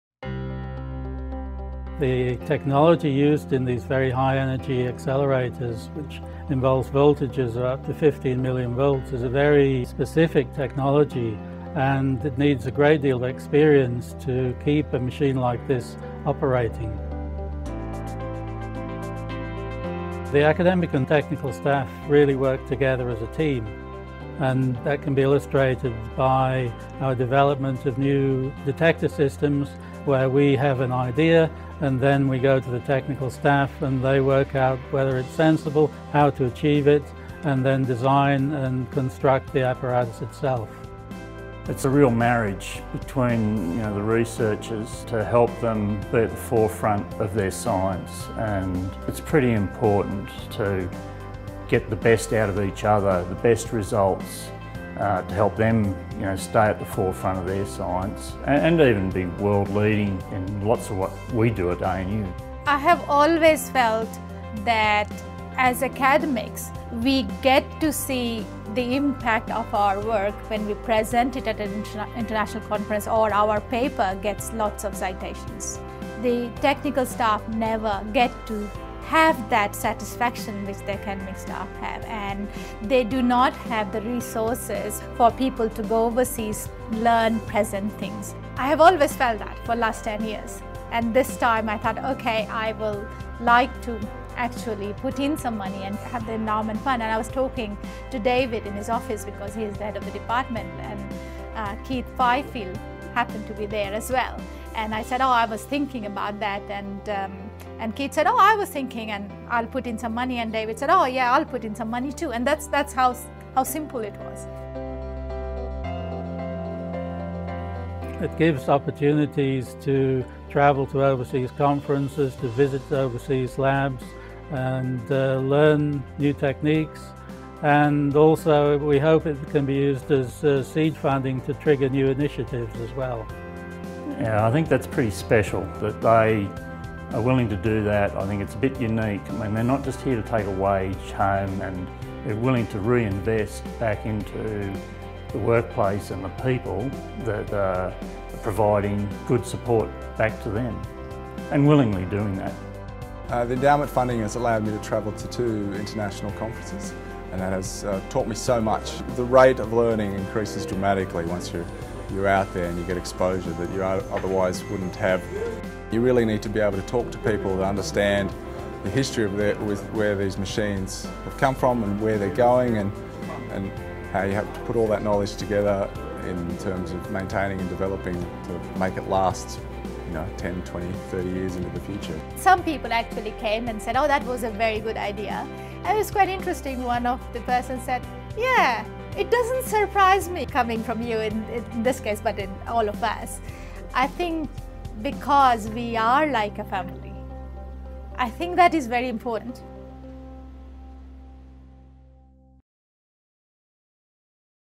ANU Nuclear Physics 'family'
Three senior members of the Department of Nuclear Physics have come together to give back to the technical staff who support their research at ANU.

They established The Heavy Ion Accelerator Facility Career Advancement Endowment.

quicklink.anu.edu.au/hiaf